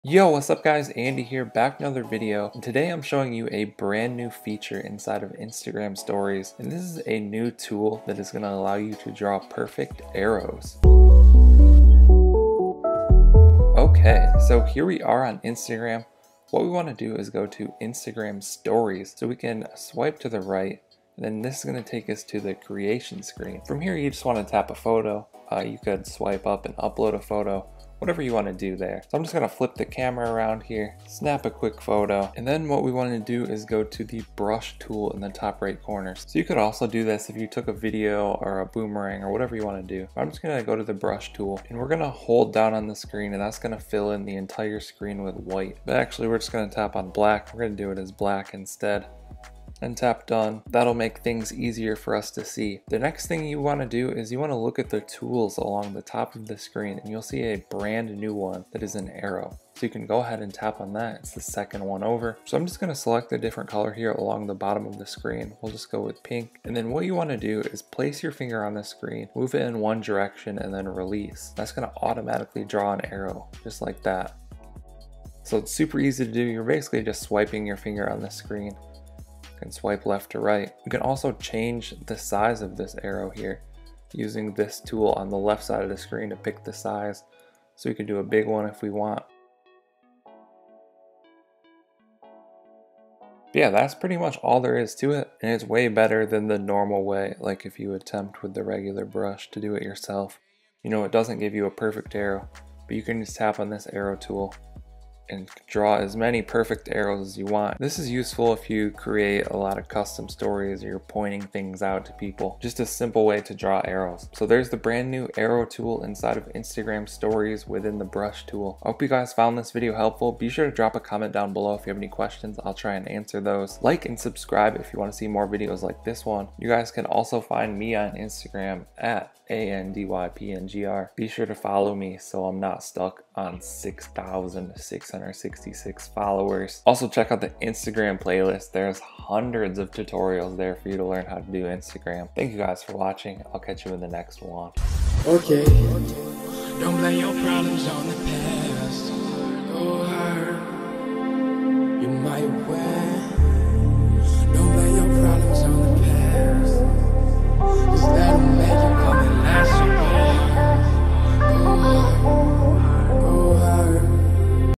How to Draw Arrows on Instagram Stories (New Feature)
How to Draw Arrows on Instagram Stories (New Feature) - A tutorial on how to draw perfect arrows with the new arrow tool in instagram stories update.